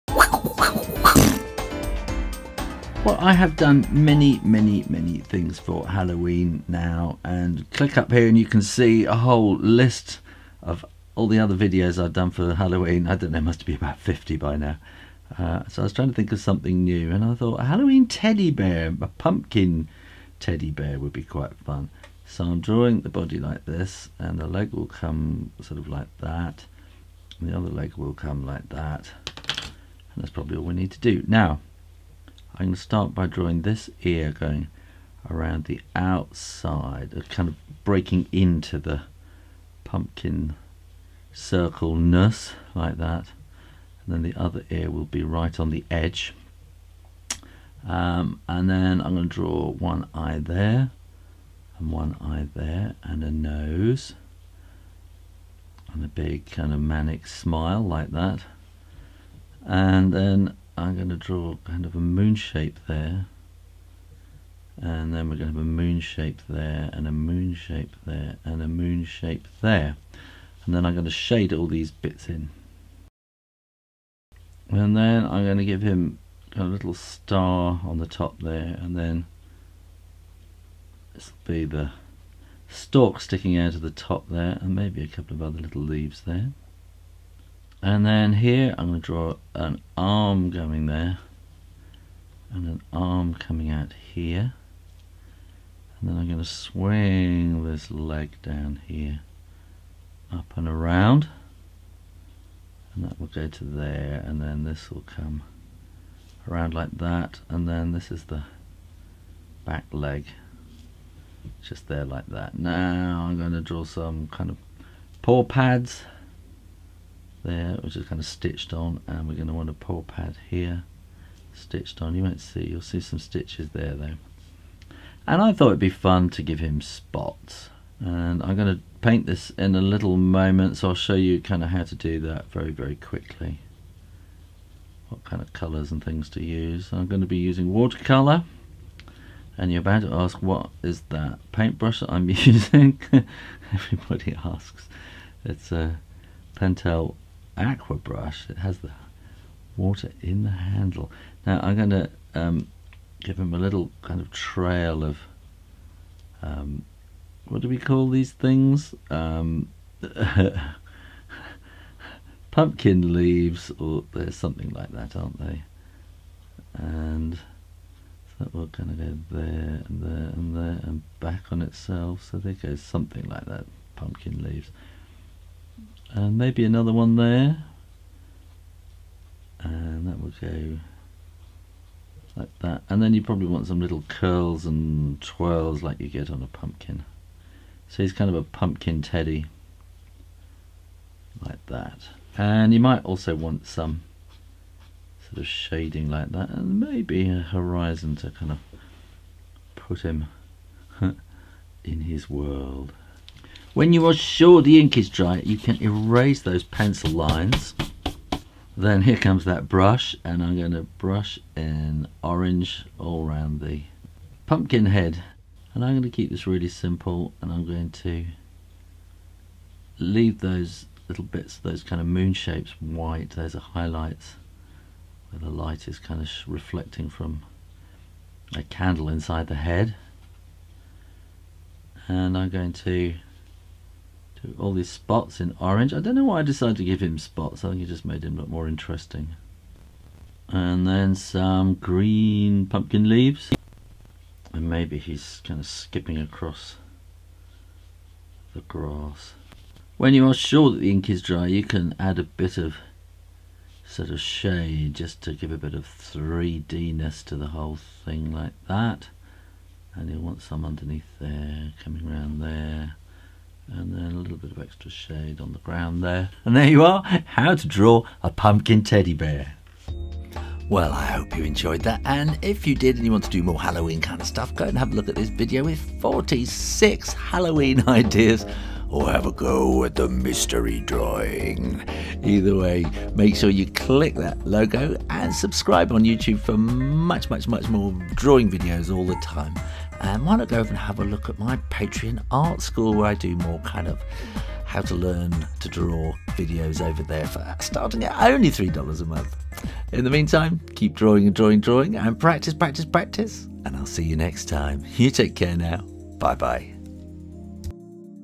How to draw a Pumpkin Teddy Bear for Halloween
Everyone asks about the tools I use when I'm out using my sketchbook. here's a video to show you what and how I use them.

The Seawhite of Brighton a5 travel journal is here in the Uk